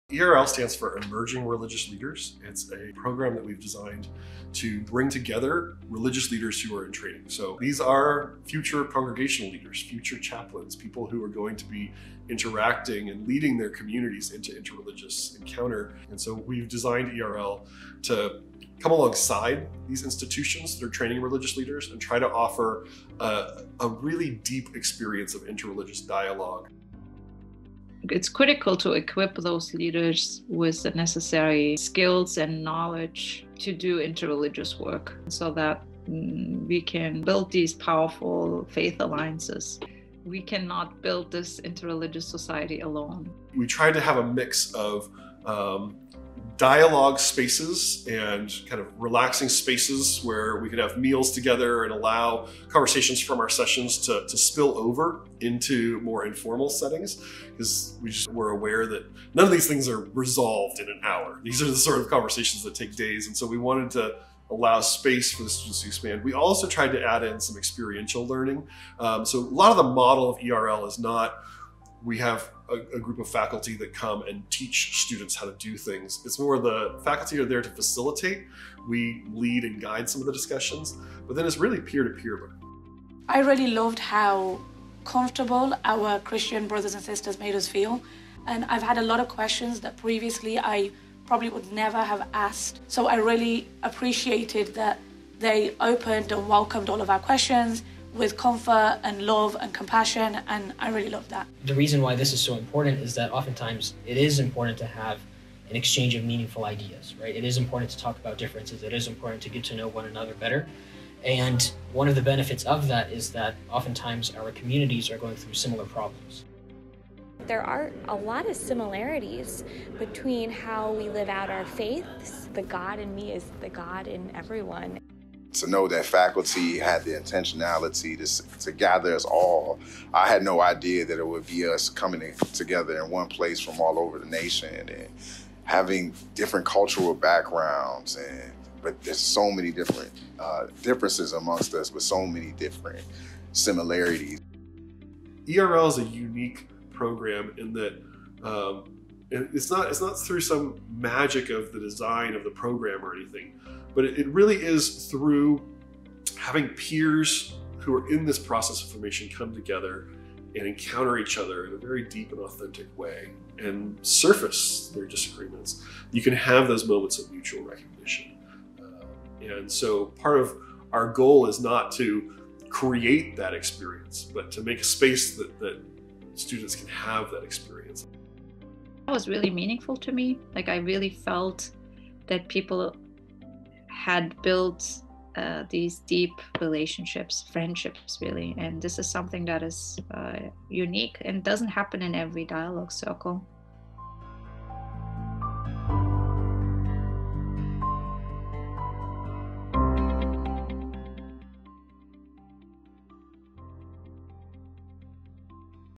ICJS Emerging Religious Leaders Course
Over five days of mutual learning, challenging discussion, and blossoming friendships, a group of more than two dozen emerging Muslim and Christian religious leaders engaged in an innovative intensive course in interreligious dialogue on June 1-5, 2022.

The Emerging Religious Leaders (ERL) course, held on the campus of Virginia Theological Seminary, is a groundbreaking program for Muslim and Christian students in the U.S. Sponsored by the Institute for Islamic, Christian, and Jewish Studies (ICJS), it was co-directed by two ICJS faculty, Protestant Scholar Matthew D. Taylor and Muslim Scholar Zeyneb Sayilgan.